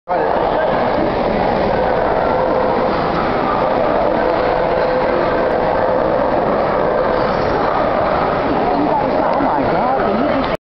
Winner's Dog Part two
Springfield IL Dog Show Afghan Classes  10/23/2010